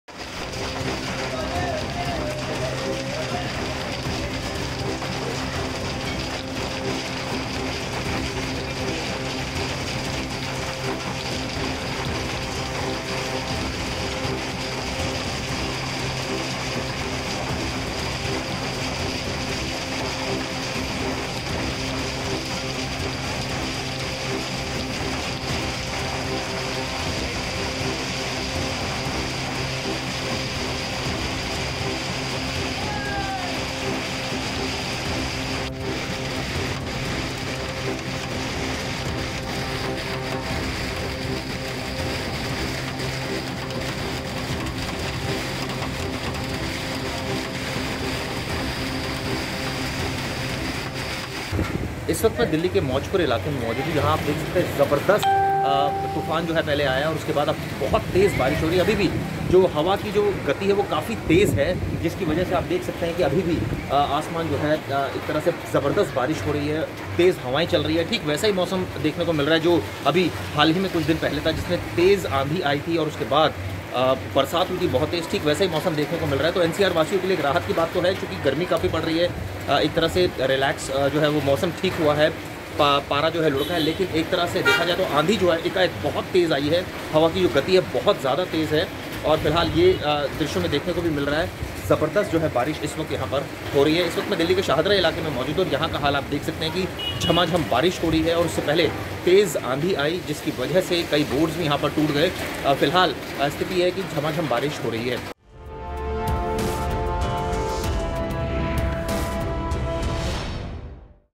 Delhi Hailstone: May महीने में दिल्ली में ये क्या हो गया?
देश की राजधानी दिल्ली में मौसम ने करवट ली है. कई इलाकों में तेज आंधी के साथ बारिश हो रही है और तेज अंधड़ के साथ ओले भी गिर रहे हैं. दिल्ली के अलावा राष्ट्रीय राजधानी में भी मौसम का मिजाज बदल गया है.